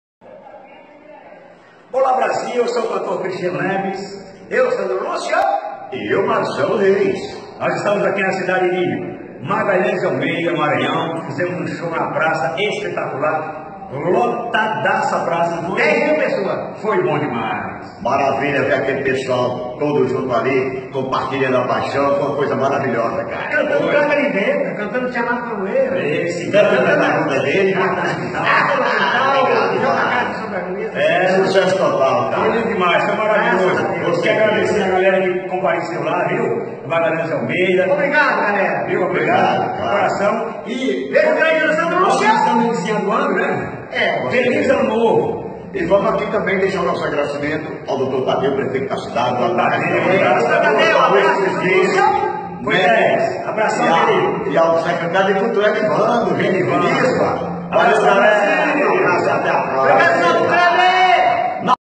Marcelo Reis, Sandro Lúcio e Cristiano Neves No Maranhão.
3 Lendas Juntas num show em Magalhães de Almeida